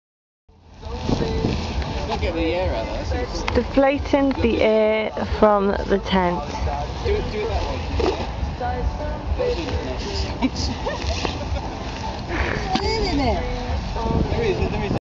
incrediblr tent rolling
Brecon Jazz 2007
Sarah rolling air out of the tent on the Rugby Club camp site, Brecon